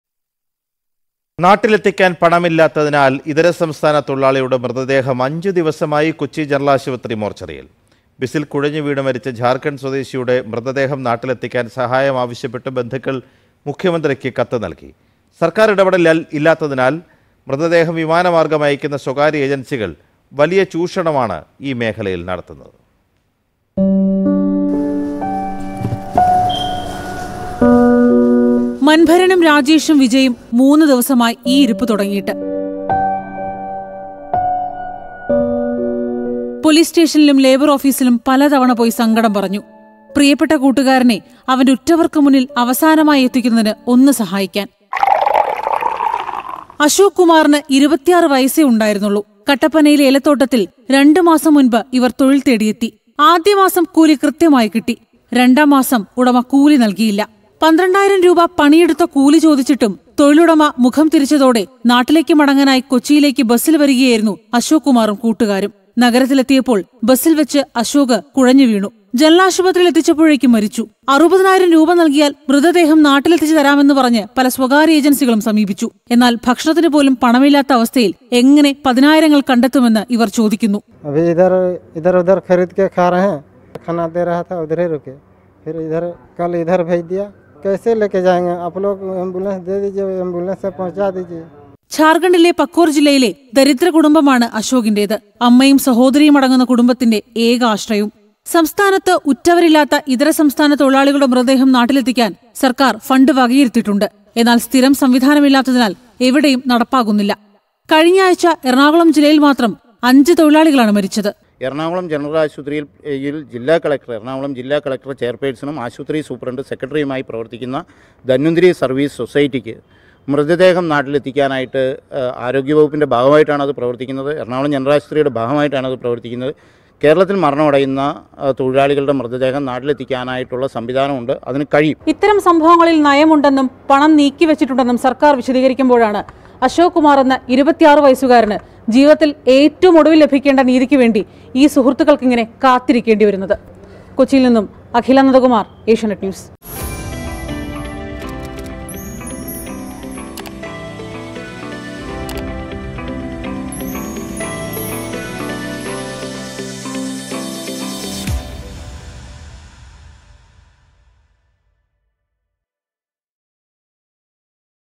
നാട്ടിലെത്തിക്കാന്‍ പണമില്ല; അതിഥി തൊഴിലാളിയുടെ മൃതദേഹം 5ദിവസമായി മോര്‍ച്ചറിയില്‍
നാട്ടിലെത്തിക്കാന്‍ പണം ഇല്ലാത്തതിനാല്‍ അതിഥി തൊഴിലാളിയുടെ മൃതദേഹം അഞ്ച് ദിവസമായി കൊച്ചി ജനറല്‍ ആശുപത്രിയിലെ മോര്‍ച്ചറിയില്‍